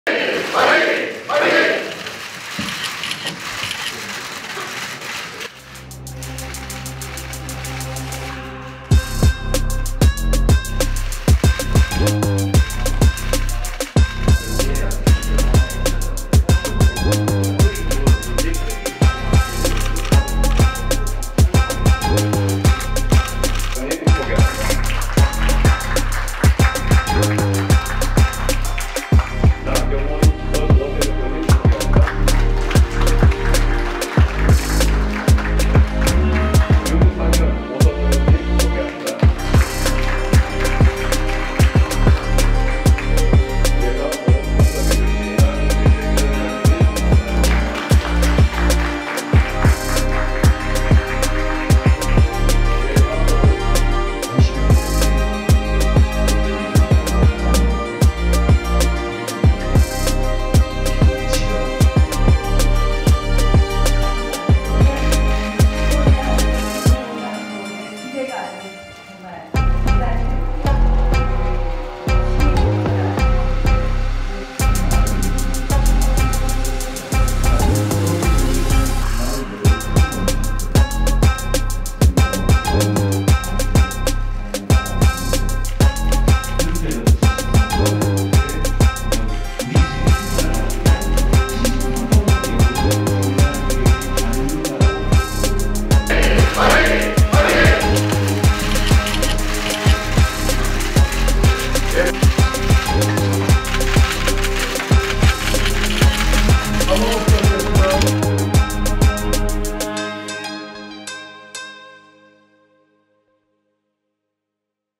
국민의힘 초선의원 공부모임에 총출동한 나경원·원희룡·윤상현·한동훈
국민의힘 당대표 출마를 선언한 나경원 의원, 원희룡 전 국토교통부 장관, 윤상현 의원, 한동훈 전 비상대책위원장이 24일 서울 여의도 국회 의원회관에서 열린 국민의힘 초선의원 첫번째 공부모임에 참석했다.

[촬영 및 편집 : 박동욱기자]